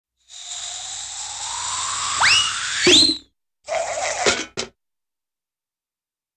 Airy Suction with Double Whistle, Plunk, Rapid, Short, Spin and Small Metallic Rattle
Airy Suction with Double Whistle, Plunk, Rapid, Short, Spin and Small Metallic Rattle · The Hollywood Edge Sound Effects Library